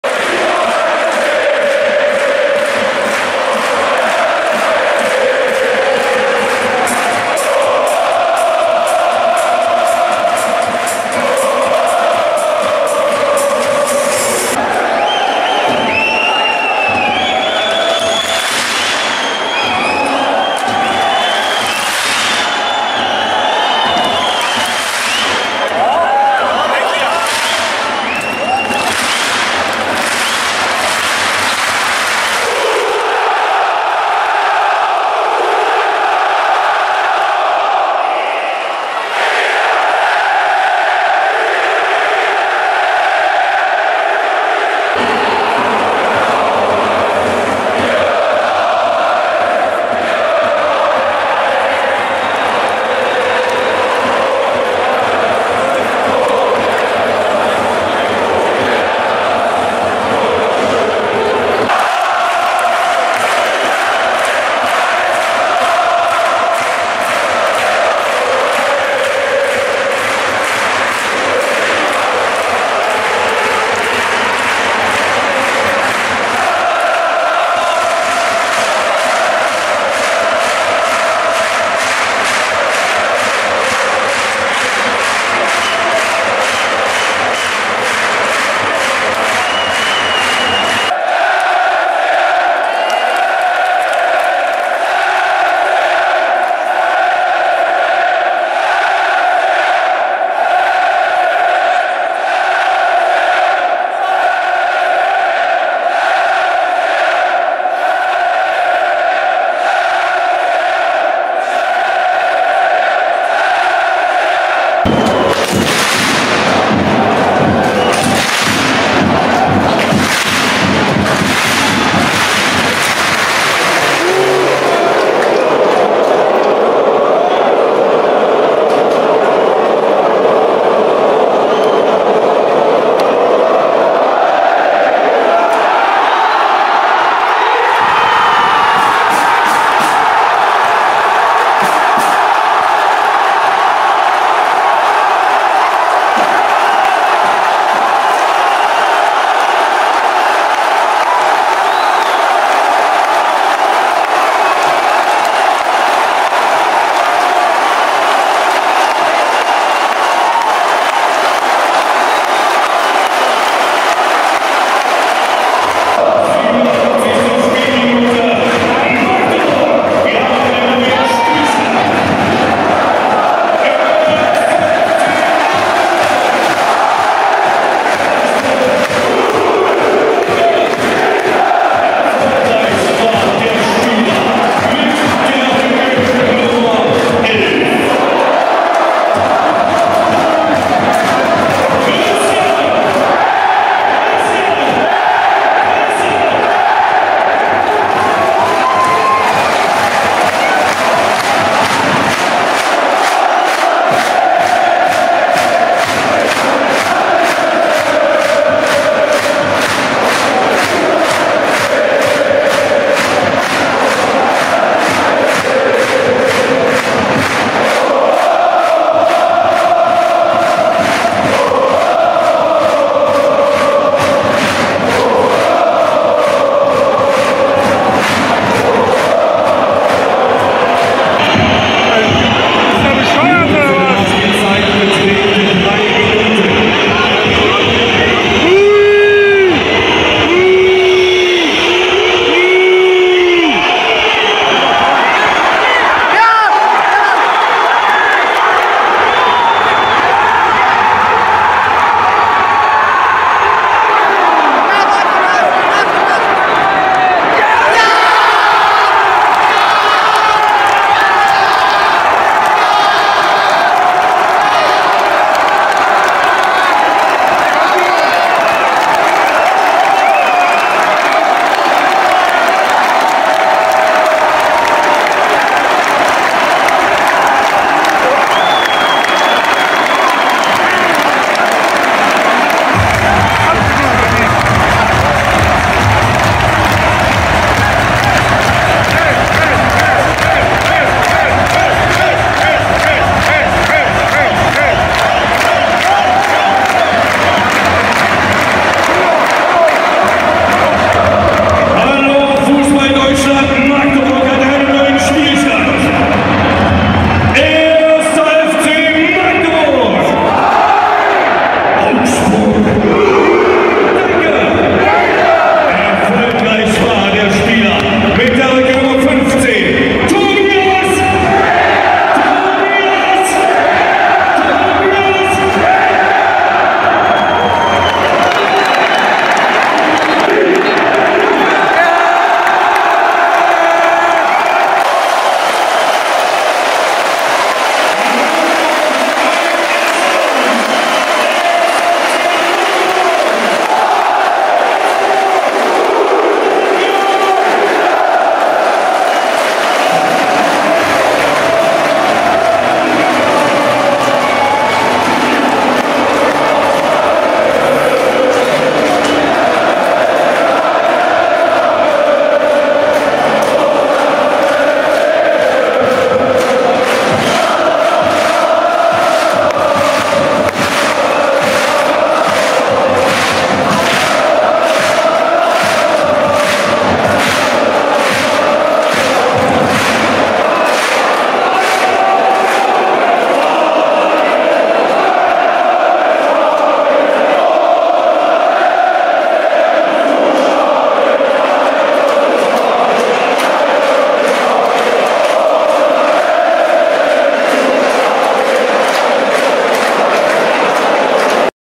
1. FC Magdeburg vs. FC Augsburg 2:0 - DFB Pokal 2017/18
Ein Zusammenschnitt des Spiels vom 1. FC Magdeburg gegen den FC Augsburg am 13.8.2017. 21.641 Zuschauer sahen das Spiel im Heinz-Krügel-Stadion (MDCC-Arena).

00:00 Vor Anpfiff
00:08 Fangesänge
01:55 Tor zum 1:0
03:51 Tor zum 2:0
05:30 Nach dem Abpfiff